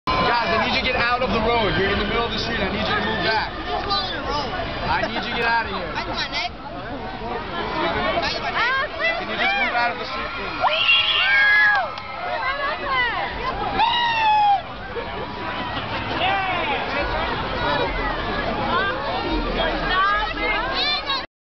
Will Smith (the Recording Of hancock)
This is  will smith's recording of hancock when me & wifey went out the saturday night before mother's day